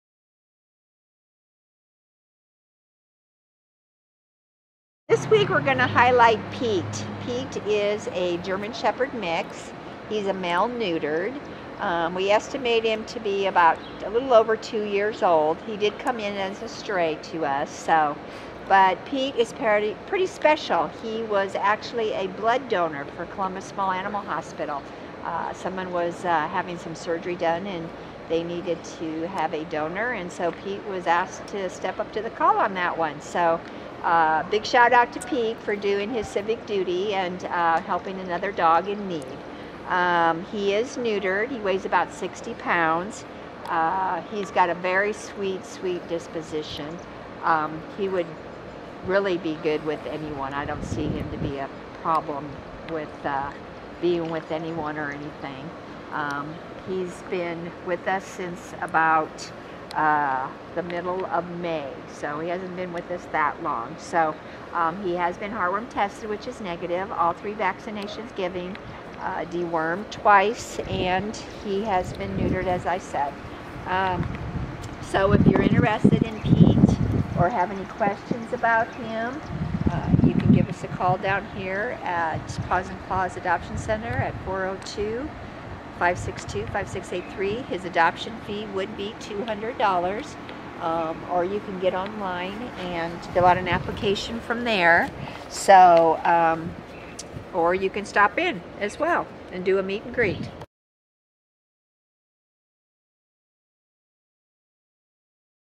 Pet of the Week for June 17: Pete
Pete, a 2-year-old German shepherd mix, is ready for adoption at the Paws and Claws Adoption Center in Columbus.